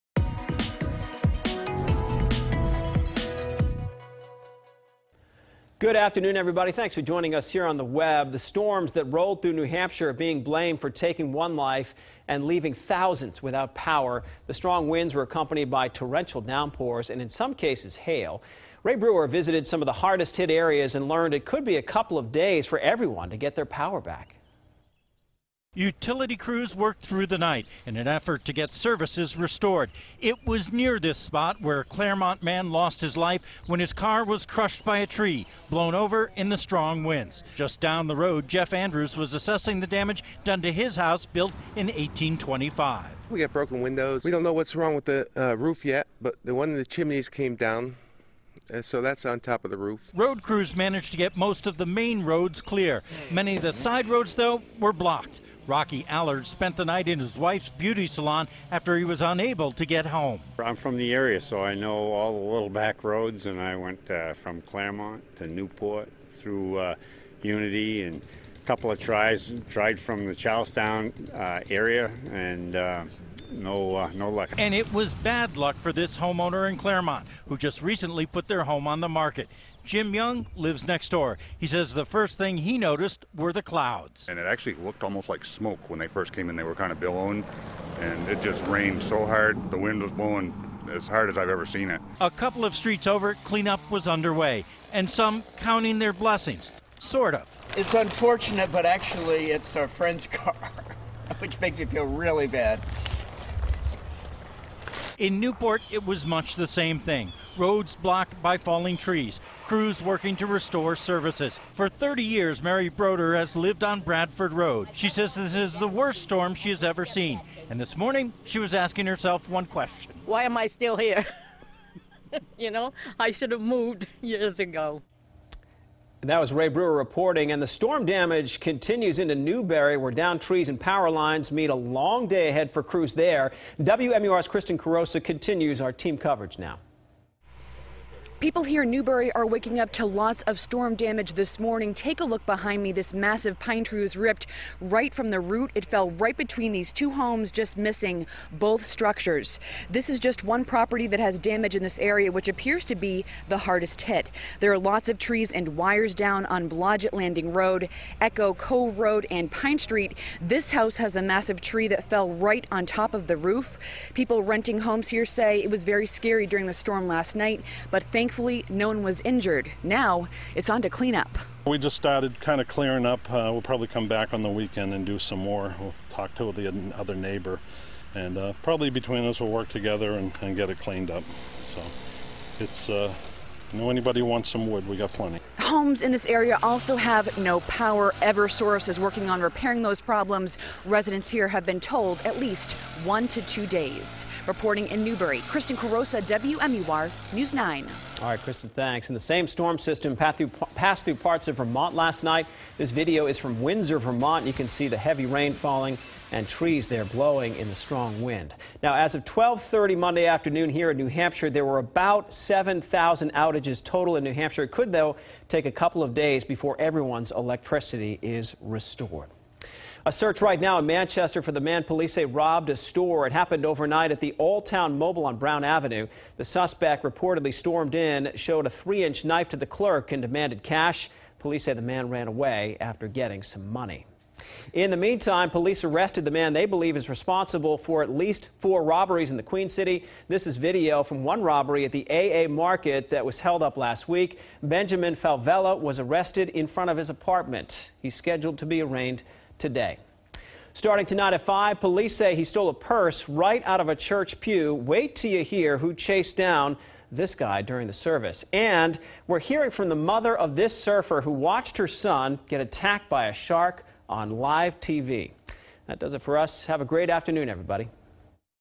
7-20-15 Afternoon news webcast
Take a look at the day's top stories with anchor Sean McDonald.